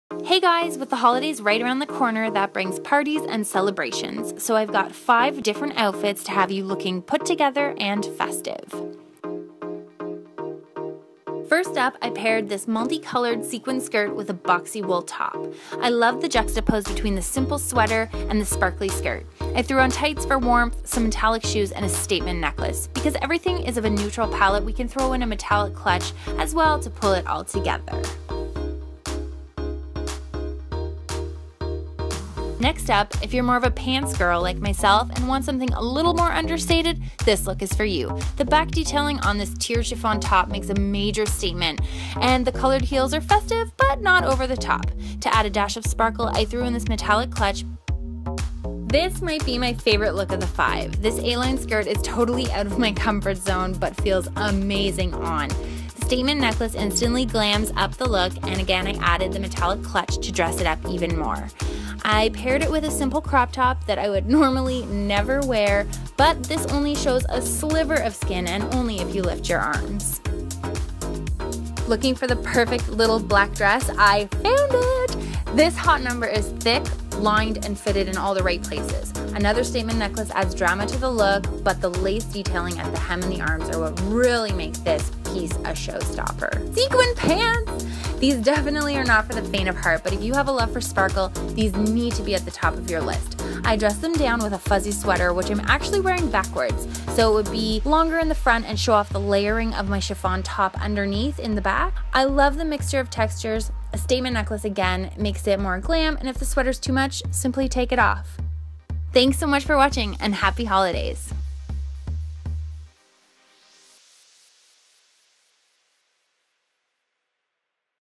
FASHION | Holiday Lookbook
OPEN ME!

Hi guys!  I'm sharing my Holiday Lookbook from December as this was a video that was initially posted on the Place d'Orleans channel and I shared on my blog and Twitter but I always, eventually, post them here to :)

Hope you're all doing well, I've got some exciting videos lined up for this new year and will see you soon :)

MUSIC

CAMERA / EDITING INFO

VLOG CAMERA | iPhone 5 & iPad mini
BEAUTY / FASHION CAMERA
EDITING SOFTWARE | Adobe Premiere Elements 11